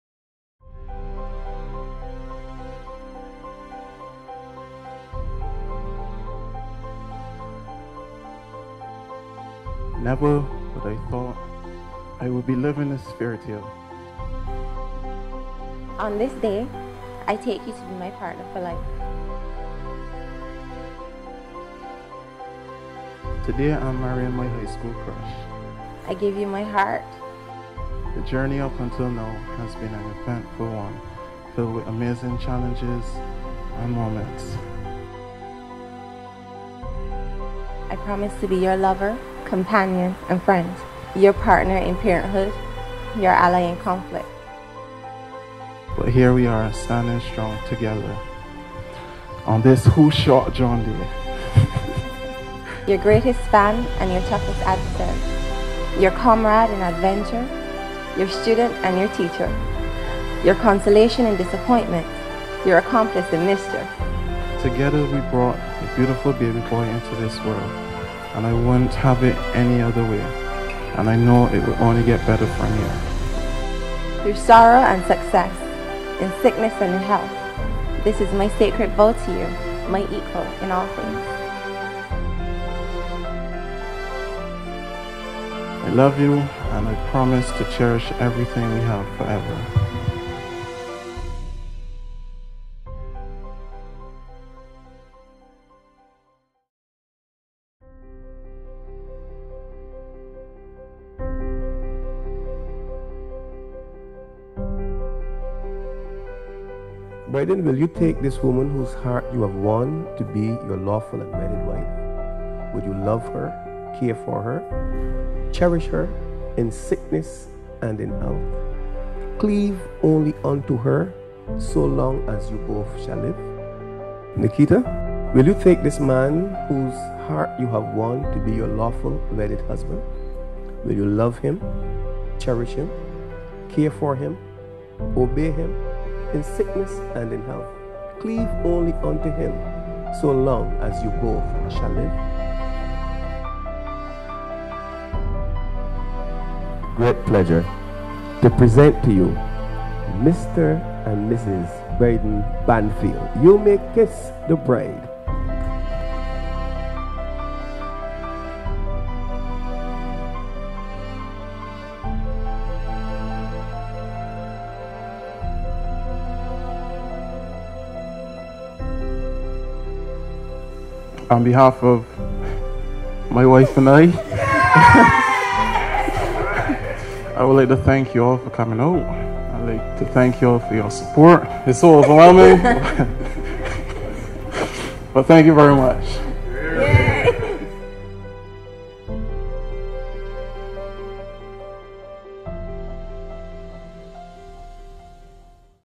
The Banfields - Wedding Day
Highlights of Wedding Ceremony.
Bryden & Nikita Banfield
Sept 30th 2017
The Bushes.